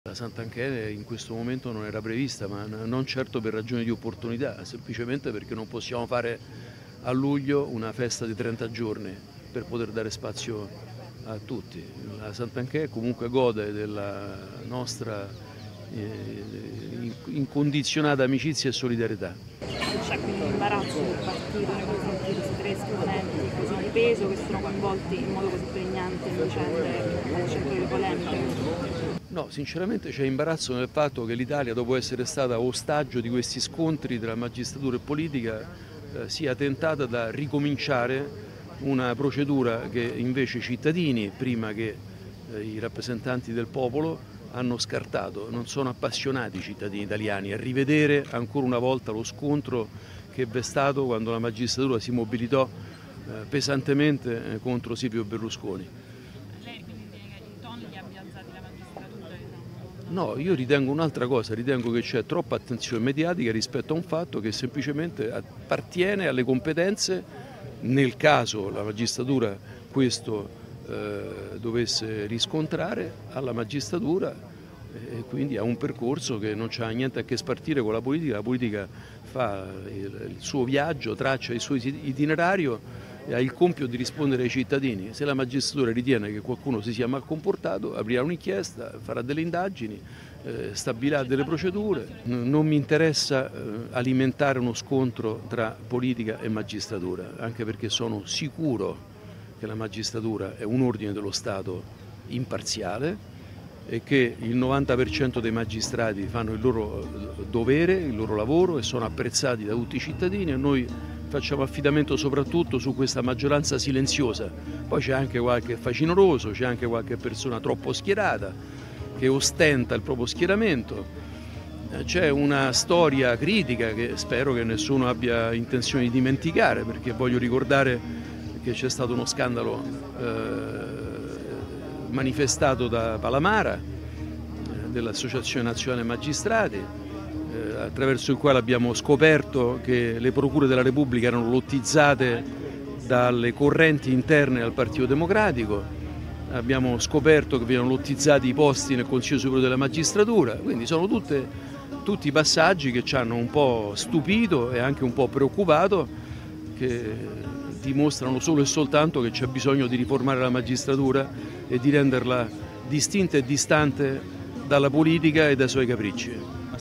Magistratura, Rampelli: "90% fanno loro dovere, poi ci sono dei facinorosi troppo schierati"
Il deputato di FdI: "Scandalo Palamara ci dice che c'è da riformare la magistratura"

POLITICA (Roma). "Santanchè gode della nostra incondizionata amicizia e solidarietà. C'è imbarazzo nel fatto che l'Italia dopo essere stata ostaggio di questi scontri tra magistratura e politica sia tentata di ricominciare una proceduta che i cittadini hanno scartato. Non mi interessa alimentare uno scontro, anche perché sono sicuro che la magistratura è un ordine dello Stato imparziale e che il 90% dei magistrati fanno il loro dovere e sono apprezzati da cittadini, facciamo affidamento su questa maggioranza silenziosa. Poi c'è qualche facinoroso, qualche persona troppo schierata che ostenta il proprio schieramento. Voglio ricordare che c'è stato uno scandalo, manifestato da Palamara, attraverso il quale abbiamo scoperto che le procure della Repubblica erano lottizzate dalle correnti interne del Partito Democratico. C'è bisogno di riformare la magistratura". Così Fabio Rampelli, deputato di FdI, a margine della festa del partito 'Piazza Italia' a Piazza Vittorio Emanuele. (Marco Vesperini/alanews)

la Santanchè in questo momento non era prevista ma non certo per ragioni di opportunità semplicemente perché non possiamo fare a luglio una festa di 30 giorni per poter dare spazio a tutti la Santanchè comunque gode della nostra incondizionata amicizia e solidarietà No sinceramente c'è imbarazzo nel fatto che l'Italia dopo essere stata ostaggio di questi scontri della magistratura e politica sia tentato da ricominciare una procedura che invece cittadini prima che i rappresentanti del Popolo hanno scartato non sono appassionati cittadini italiani e ancora una volta lo scontro che è stato quando la magistratura si mobilitò pesantemente contro Silvio Berlusconi  No io ti tengo un'altra cosa Ritengo che c'è troppa attenzione mediatica rispetto a un fatto che semplicemente appartiene alle competenze nel caso la magistratura questo riscontrare alla magistratura e quindi ha un percorso che non c'ha niente a che spartire con la puoi fare il suo viaggio traccia i suoi itinerario e hai di rispondere cittadini Se la magistratura ritiene che qualcuno si chiama comportato Avvia un'inchiesta farà delle indagini stabilire delle procedure non mi interessa alimentare uno scontro tra politica e magistratura Anche perché sono sicuro che la magistratura è un ordine dello Stato imparziale e che il 90% dei magistrati fanno il loro lavoro e sono apprezzati da tutti i cittadini e noi facciamo affidamento soprattutto su questa maggioranza silenziosa poi c'è anche qualche facinoroso c'è anche qualche persona troppo schierata che ho Stan  schieramento e c'è una storia critica che spero che nessuno abbia intenzione di dimenticare perché voglio ricordare che c'è stato uno scandalo manifestato da Palamara dell'Associazione Nazionale magistrati attraverso il quale abbiamo scoperto che le procure della Repubblica non utilizzate dalle correnti interne al partito democratico abbiamo scoperto che vengono utilizzati i posti nel consiglio della magistratura quindi sono tutte tutti i passaggi che c'hanno un po' stupido e anche un po' preoccupato che dimostrano solo e soltanto che c'è bisogno di riformare la magistratura e di renderla distinto è distante dalla politica e dai suoi capricci